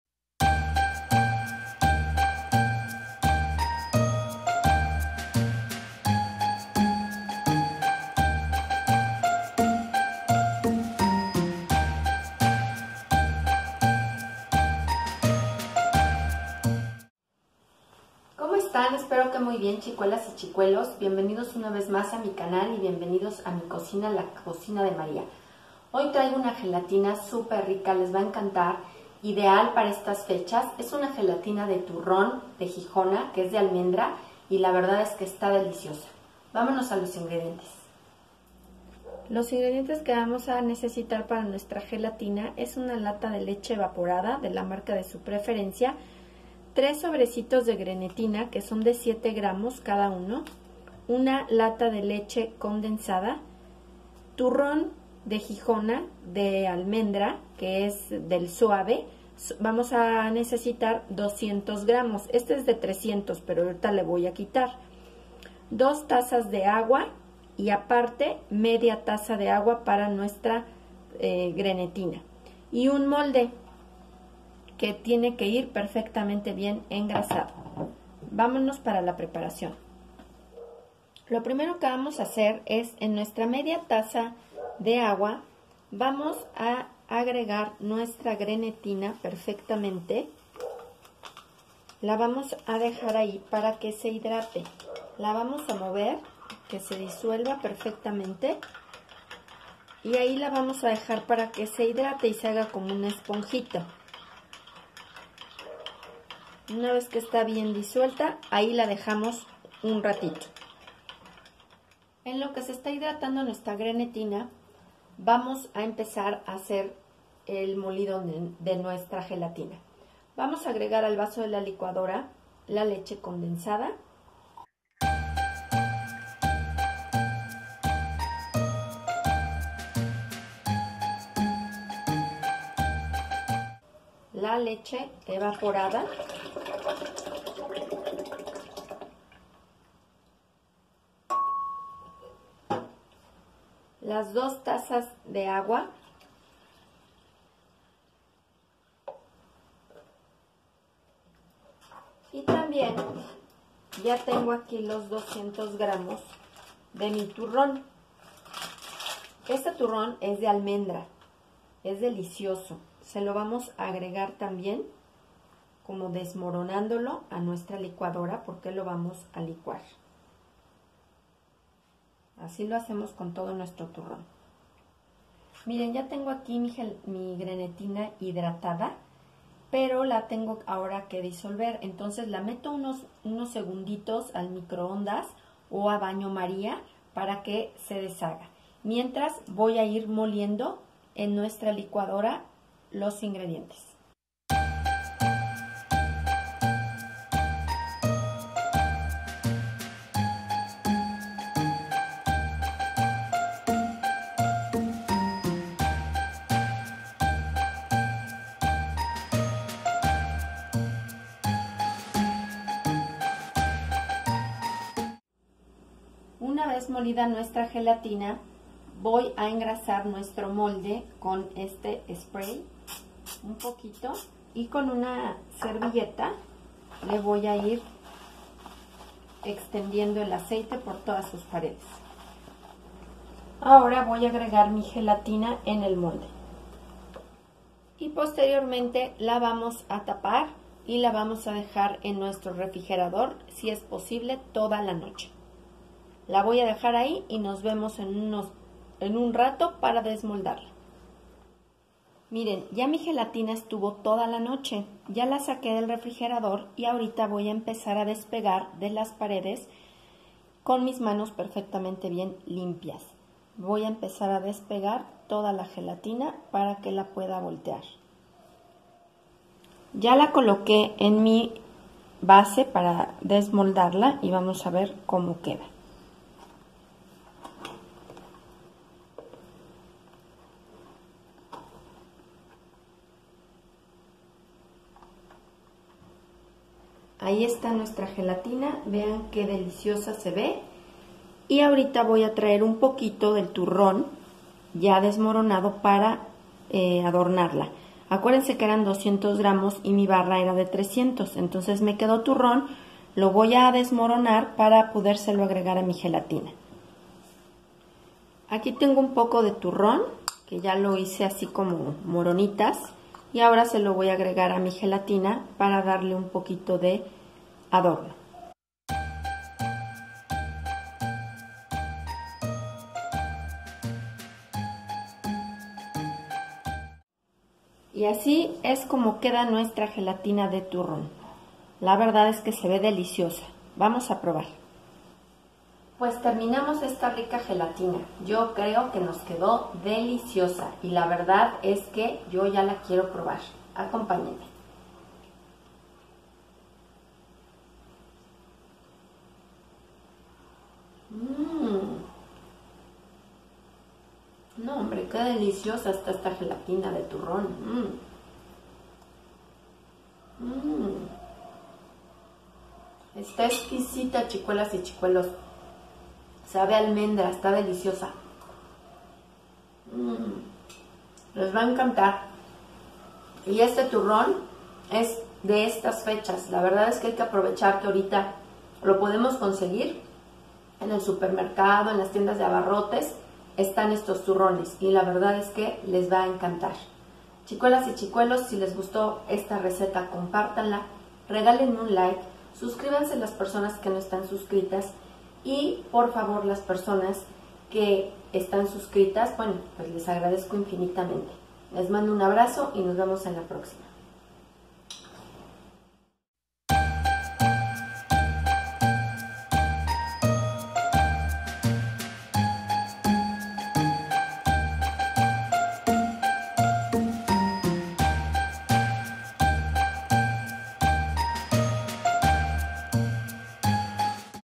Receta de GELATINA DE TURRÓN :) fácil de preparar!
Los invito a preparar esta gelatina de turrón de almendras, queda deliciosa y es muy fácil de preparar, ideal para las fiestas de diciembre.

Ingredientes:

1 Lata de leche evaporada
1 Lata de leche condensada
3 Sobres de grenetina de 7 gr c/u
200 gr Turrón de almendra blando
2 Tazas de agua
1/4 Taza de agua para hidratar nuestra grenetina